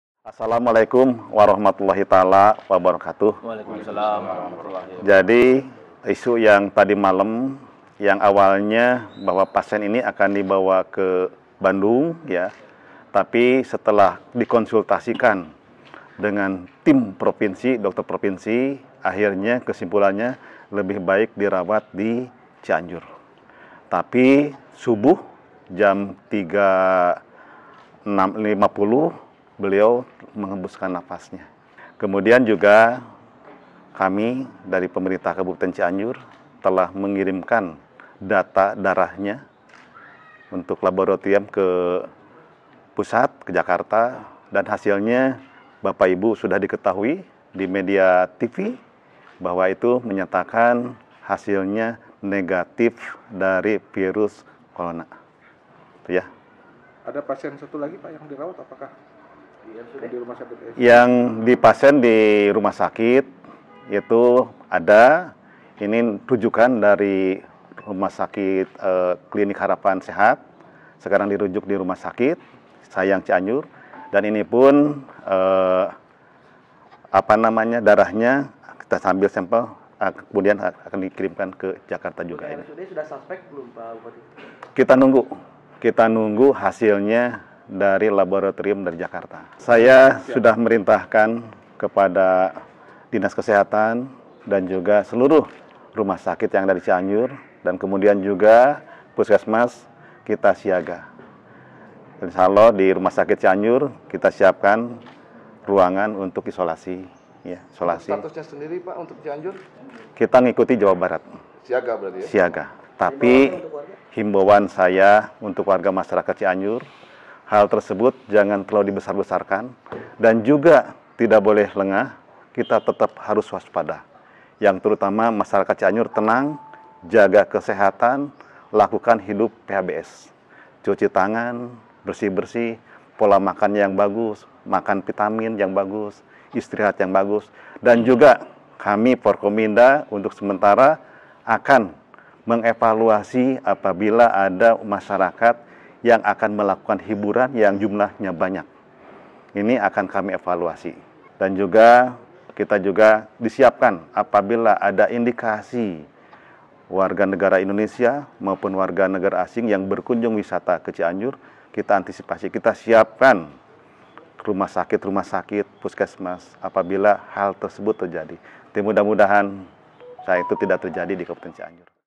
Cianjur Waspada Corona
INFOCIANJUR – Plt Bupati Cianjur H. Herman Suherman pastikan pasien meninggal suspect Virus Corona (COVID-19) Negatif dari Virus Corona.

“Pasien yang semalam dirawat di RSDH itu awalnya akan dirujuk ke RSHS Bandung setelah dikonsultasikan bersama Dokter dari Provinsi kesimpulannya dirawat di Cianjur, tapi saat Subuh beliau menghembuskan nafas terakhir,” kata Plt Bupati Cianjur H. Herman Suherman saat Konferensi Pers di Pendopo Pemkab Cianjur, Selasa (03/03)

Herman mengatakan, Pemerintah daerah Kabupaten Cianjur sebelumnya telah mengirimkan sample darah pasien tersebut ke Pusat dan hasilnya negatif dari Virus Corona.